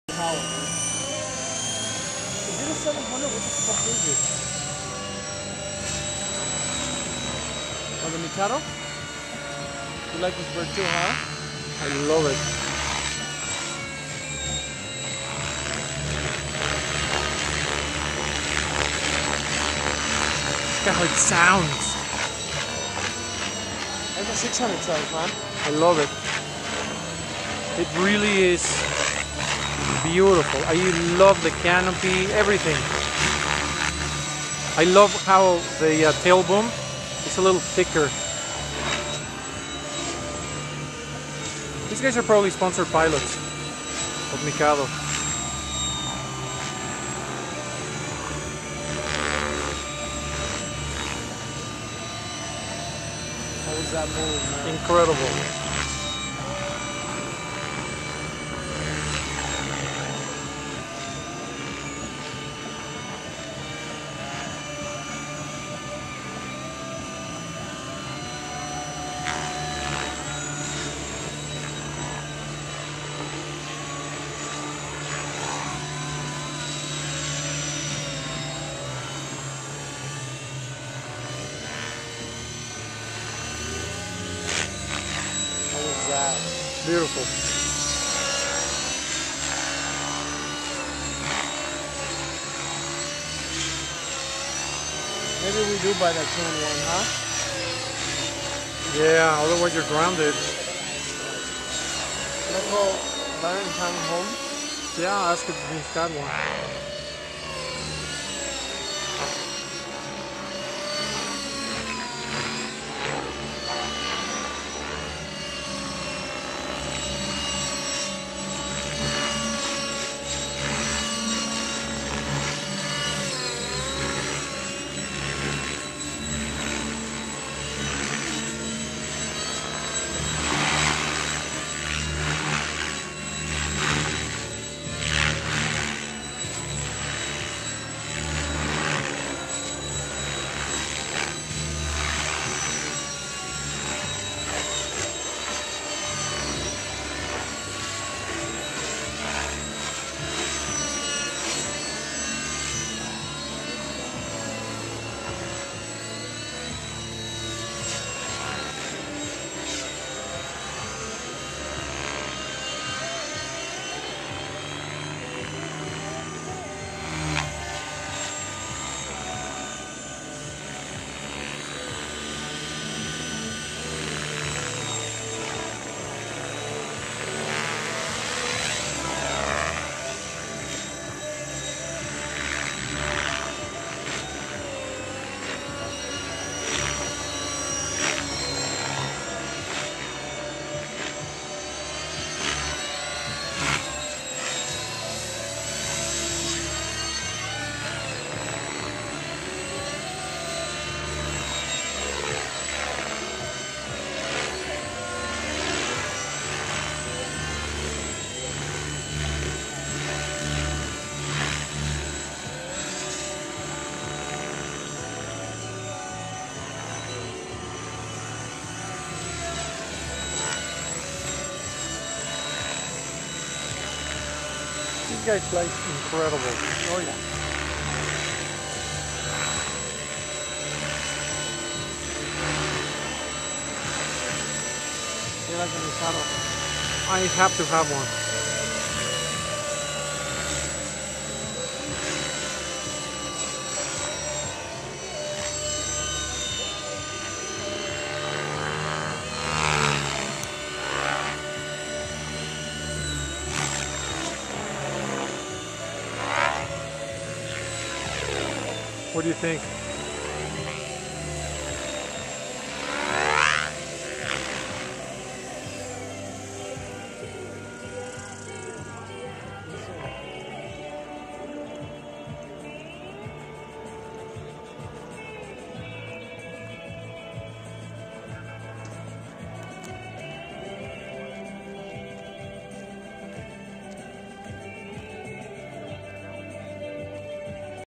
Team Mikado - 600 - SVF Heli Fun Fly - 02-26-10 Friday
I don't know this pilot's name, but he flies incredible.
And his helicopter was insane!
Captured this footage at Sun Valley Fliers on Friday February 26th 2010 at the Helicopter Fun Fly